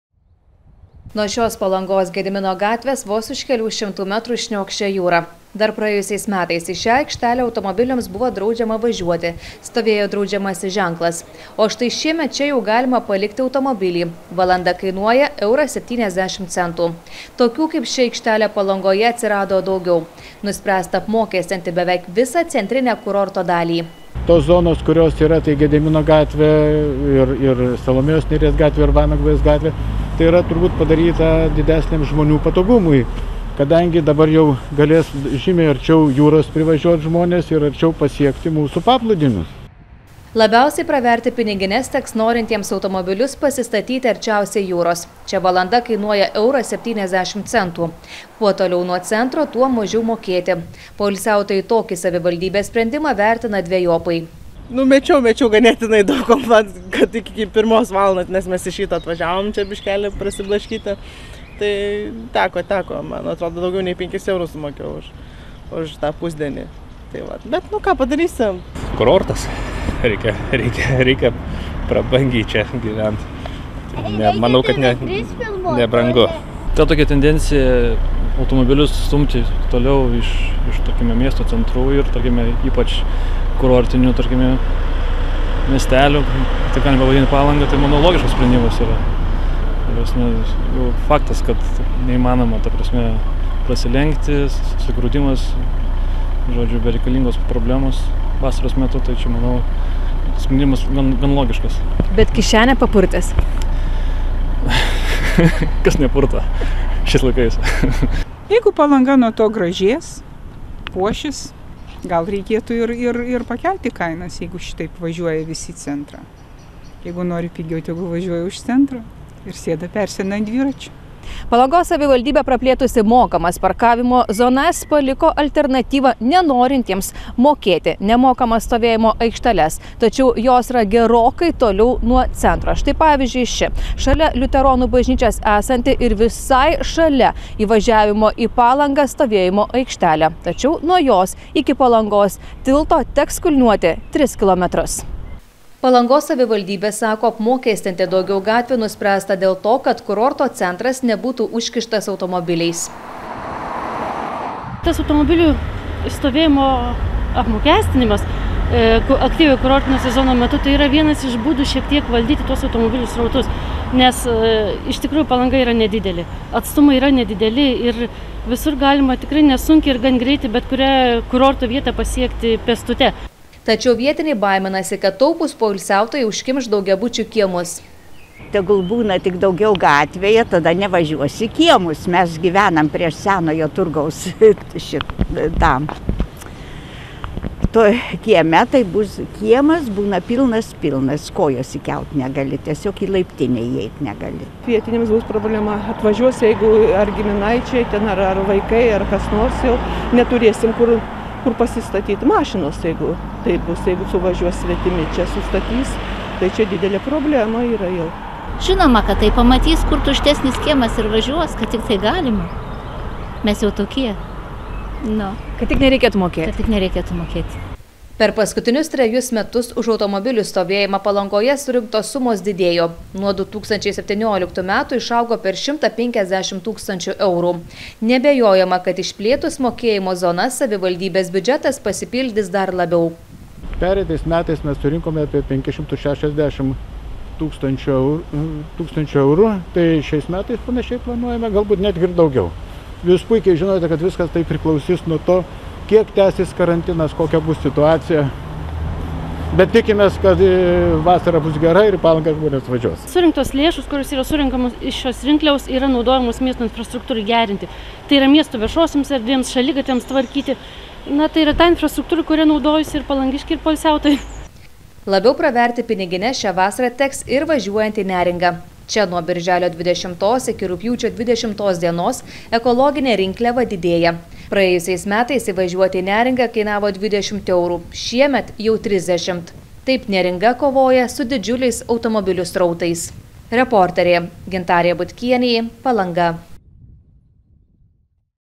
Poilsiautojams pajūryje teks patuštinti pinigines: nebeliks nemokamų stovėjimo aikštelių
Šiemet į kurortus atvykstantiems poilsiautojams teks labiau praverti pinigines. Palangos savivaldybė automobilių statymą apmokestino, o zonas praplėtė – teks mokėti už automobilio statymą beveik visoje centrinėje kurorto dalyje. Nemokamai automobilius bus galima statyti tik aikštelėse, kurios nuo jūros yra už kelių kilometrų. Vietiniai baiminasi, kad poilsiautojai taupydami užkimš daugiabučių kiemus. O štai Neringa 10 eurų padidino ekologinį įvažiavimą. Taip kurortai kovoja su didžiuliais automobilių srautais.